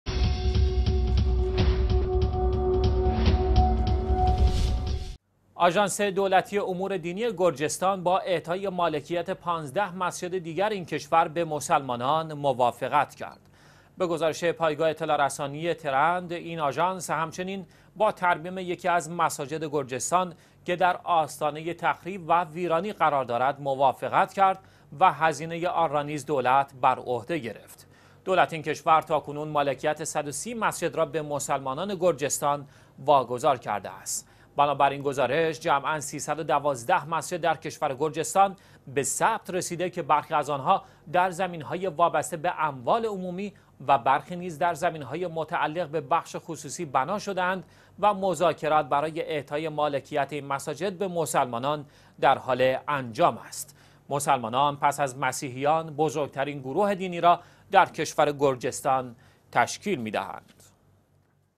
اعطای مالکیت ۱۵ مسجد، به مسلمانان گرجستان / ۱۳ شهریور ۱۳۹۵
اعطای مالکیت ۱۵ مسجد، به مسلمانان گرجستان

آژانس دولتی امور دینی گرجستان، با اعطای مالکیت ۱۵ مسجد دیگر این کشور به مسلمانان موافقت کرد.

به گزارش پایگاه اطلاع‌رسانی «trend» این آژانس همچنین، با ترمیم یکی از مساجد گرجستان که در آستانه تخریب و ویرانی قرار دارد، موافقت نمود و هزینه آن را نیز دولت بر عهده گرفت.

دولت این کشور تاکنون مالکیت ۱۳۰ مسجد را به مسلمانان گرجستان واگذار کرده است.

بنابراین گزارش، به طور کلی ۳۱۲ مسجد در کشور گرجستان به ثبت رسیده که برخی از آن‌ها در زمین‌های وابسته به اموال عمومی و برخی نیز در زمین‌های متعلق به بخش خصوصی بنا شده‌اند که مذاکرات برای اعطای مالکیت این مساجد به مسلمانان در حال انجام است.

مسلمانان بزرگترین گروه دینی بعد از مسیحیت را در کشور گرجستان تشکیل می دهند.